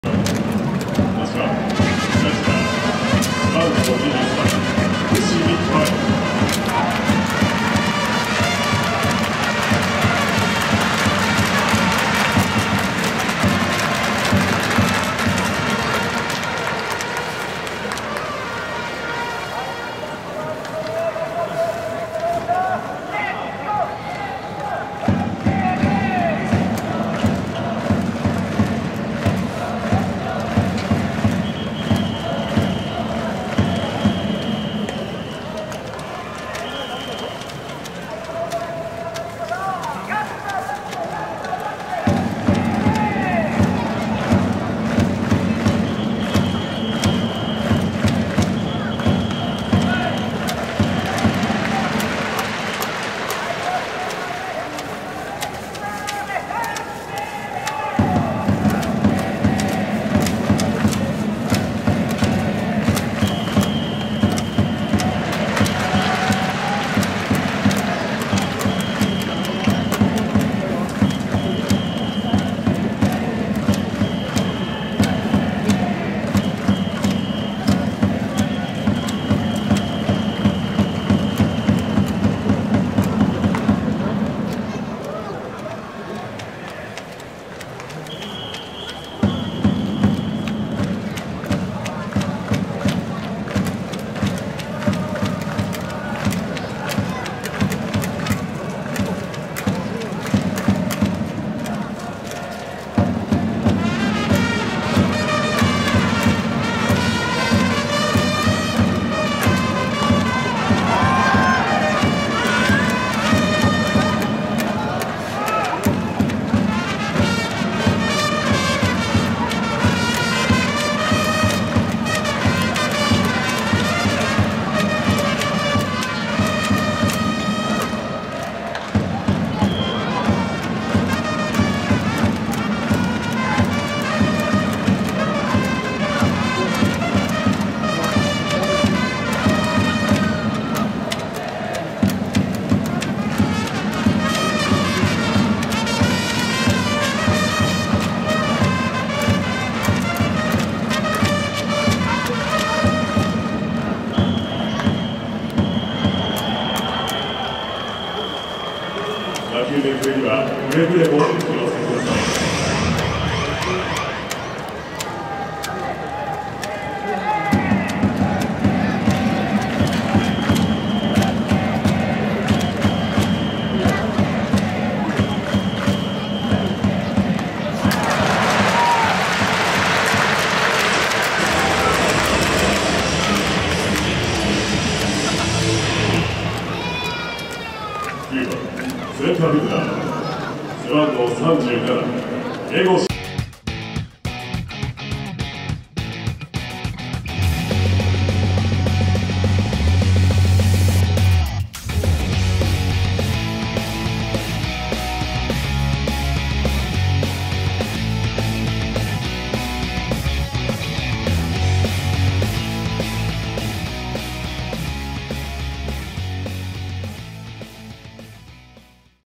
宮城大弥ＶＳ伏見寅威 元女房役と対決
伏見選手に一塁側からも拍手で迎えられる。お互いを知るバッテリー対決.